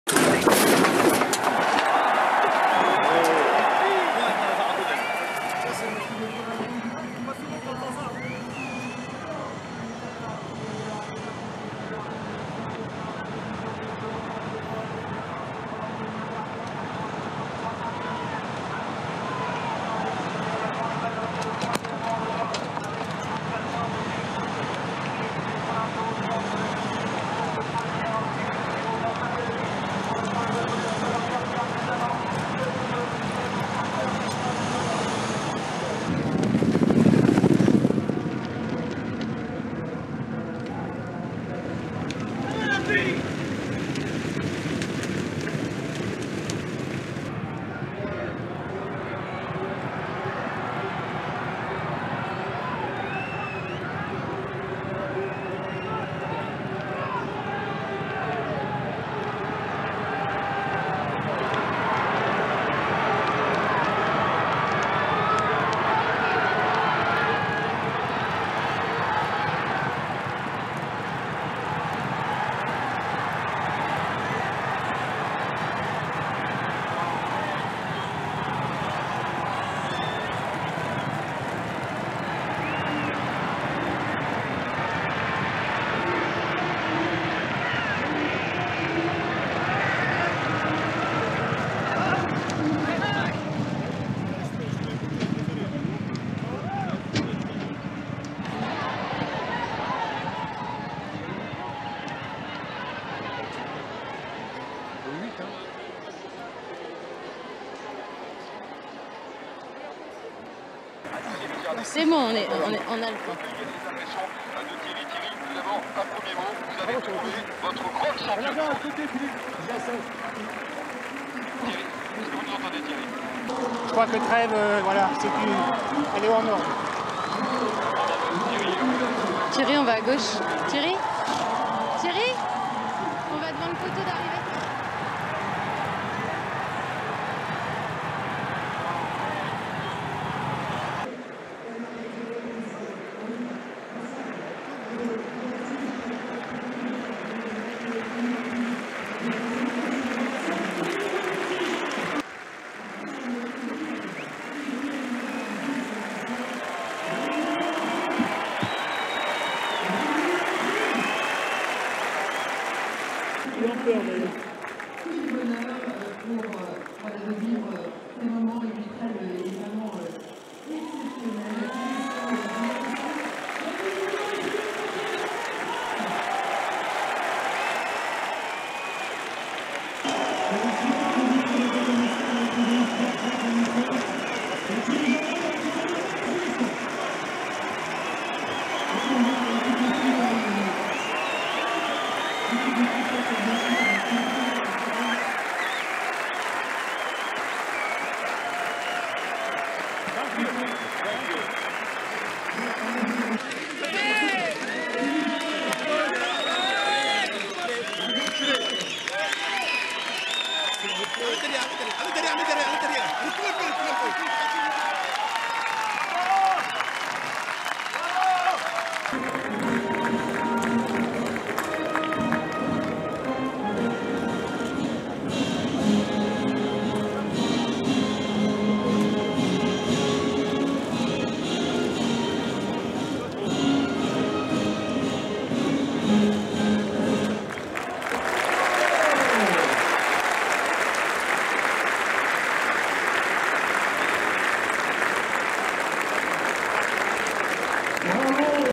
2014 Arc: Treve wins again! Full race re-run
Footage from Longchamp as Treve, ridden by Thierry Jarnet, wins her second Prix de l'Arc de Triomphe.